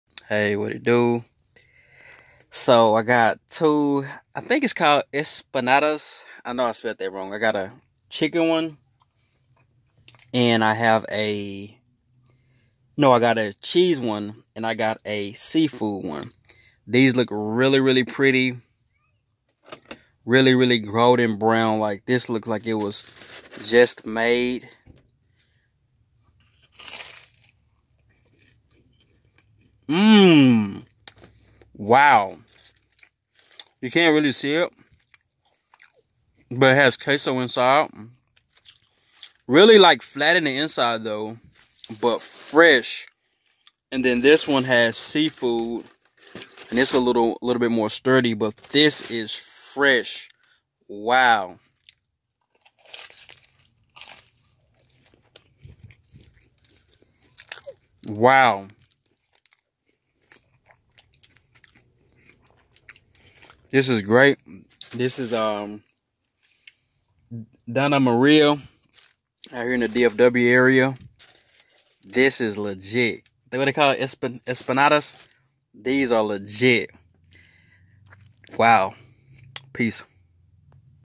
Reviewing empanadas from Dona Maria
8/10 fresh tasting, I got a cheese, and a seafood one. the seafood one was a little thicker inside the cheese one tasted good.